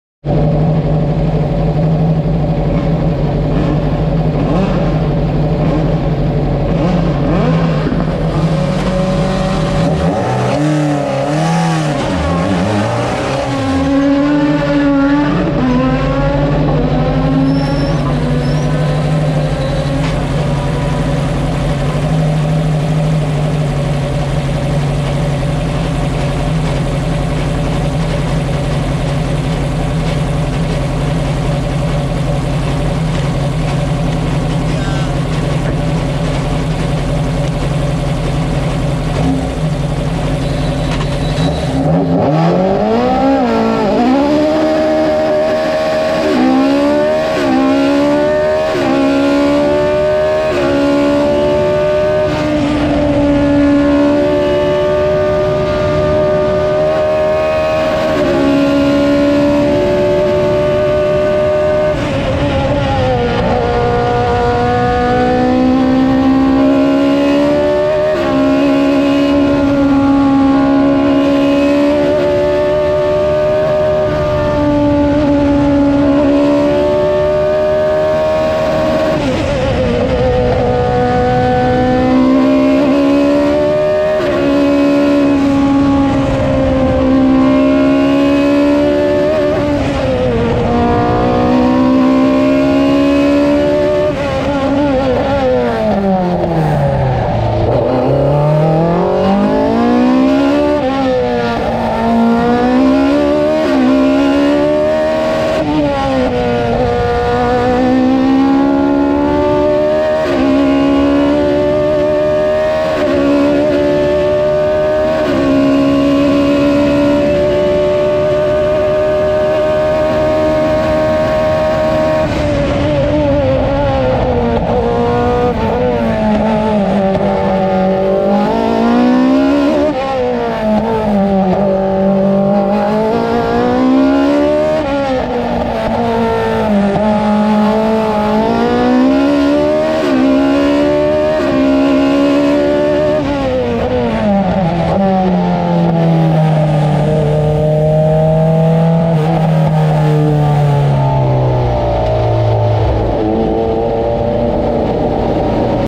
Astenersi deboli di cuore: Cameracar con Sergio Farris
A bordo con Sergio Farris su per la gola del Bottaccione 54° trofeo Luigi Fagioli, 9° prova del campionato Italiano Velocità Montagna.. Crono di 1'39"32 Con brivido al curvone che fa perdere parecchio tempo.  Si vola sul filo dei 160 Km/H  di media.

Pilota: Sergio Farris
Auto: Osella Pa2000 Evo
Scuderia: Speed Motor
Team: Dalmazia motorsport
Pneumatici: Avon
Impegnato come testimonial dell AVIS donatori sangue.